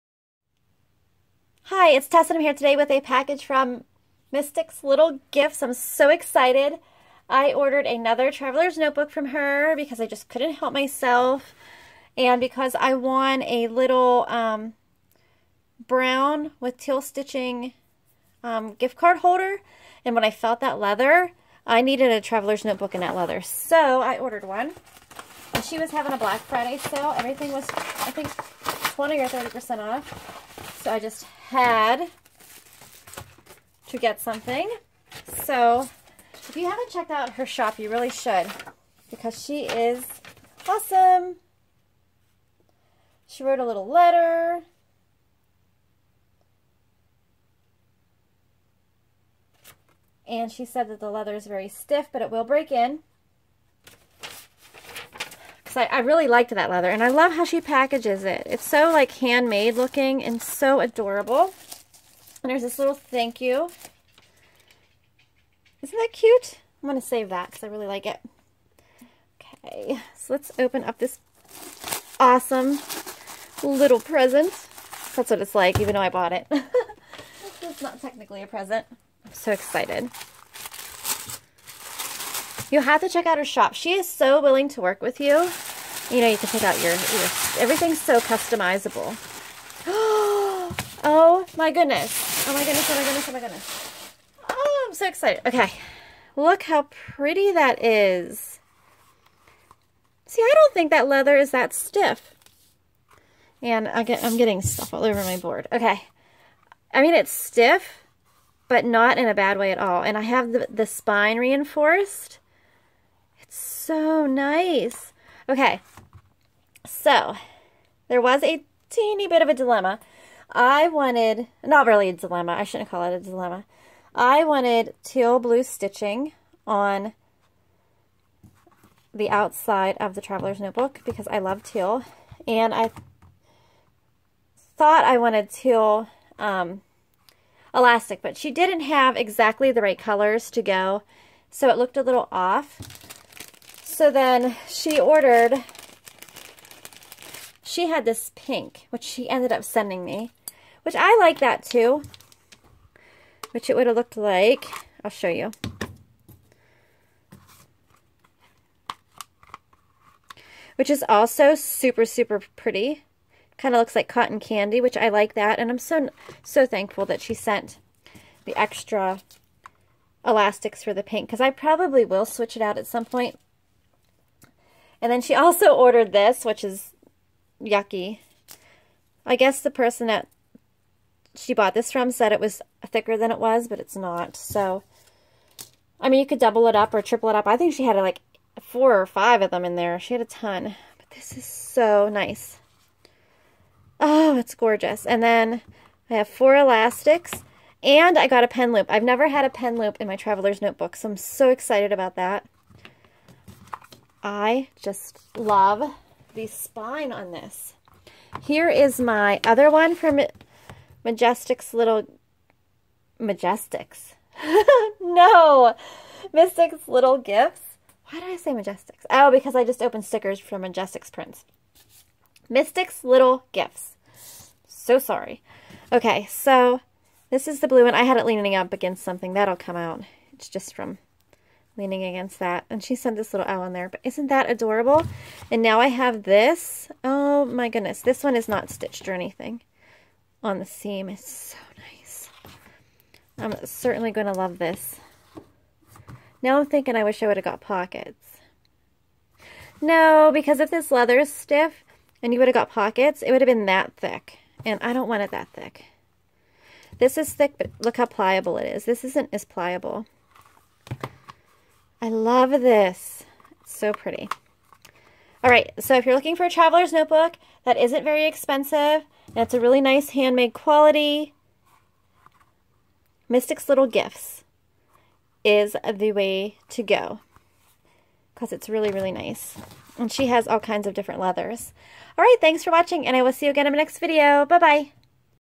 MYSTICS LITTLE GIFTS / POCKET SIZE TN / UNBOXING
My goodness...is this gorgeous! I just love how this TN looks and feels. and SMELLS! Divine.

HURRICANE RANDI STICKER CO (TESSLEEPR for $5 off $25)